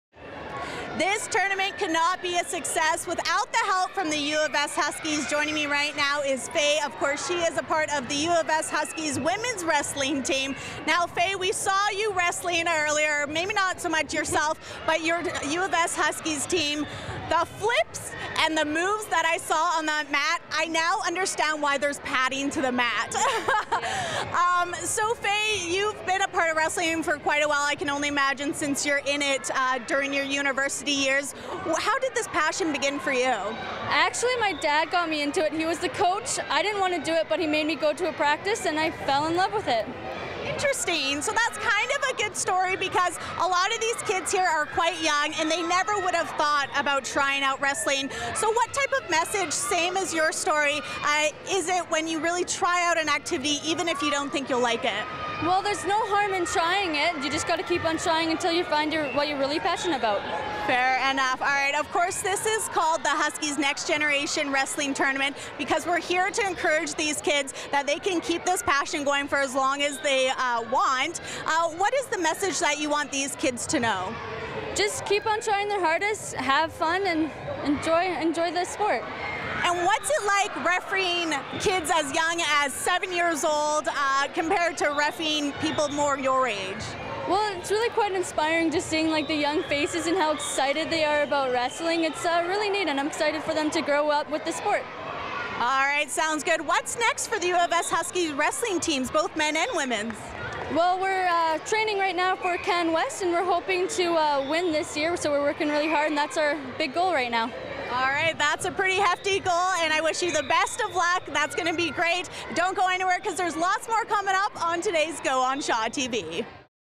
U of S Huskies Wrestling Team
The U of S Huskies’ Men & Women’s wrestling teams were at a recent tournament for young wrestlers. The Huskies hope by showing these kids where the sport of wrestling can take them, more young students will be encourage to find out what they’re passionate about.